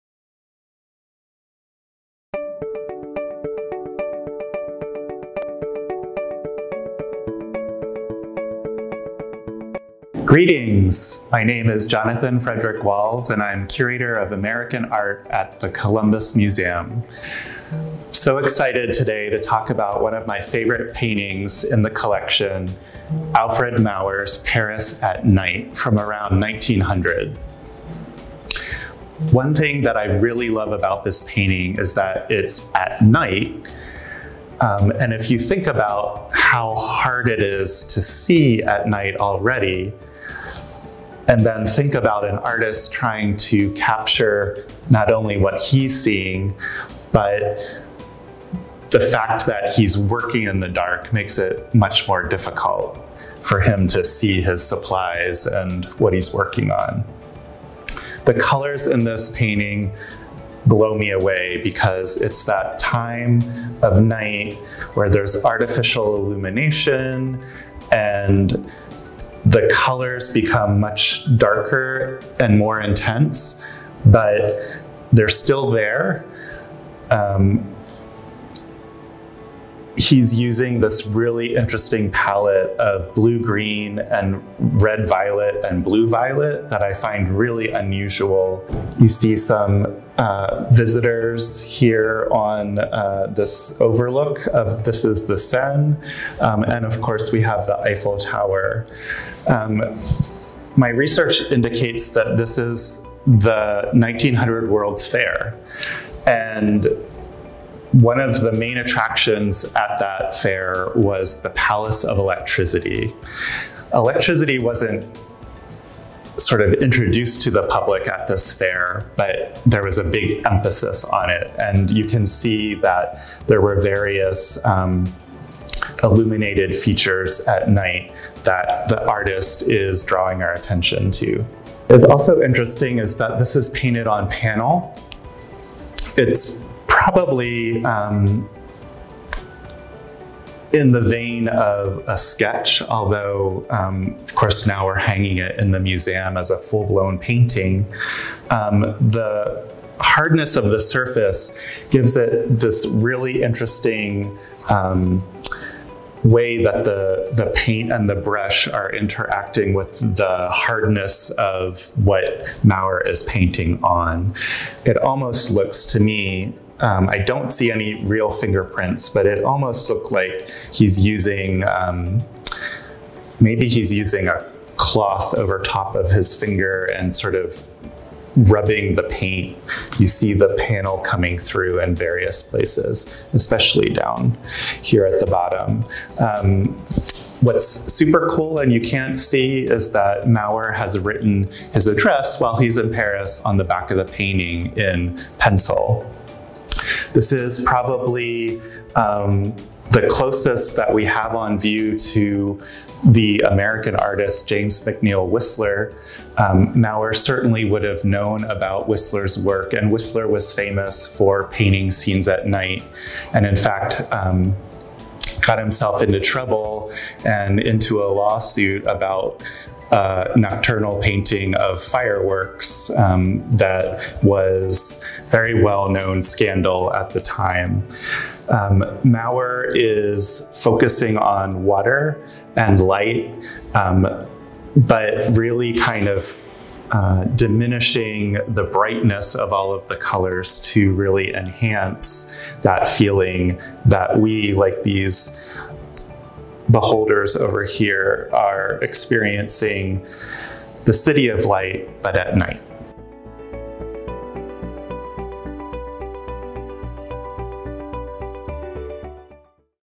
Curator Chat: Alfred Maurer's "Paris at Night"
MuseumFromHome with us as Jonathan Frederick Walz, Ph.D., Curator of American Art at The Columbus Museum, talks about one of his favorite paintings in the collection: "Paris at Night," 1900, by Alfred Maurer.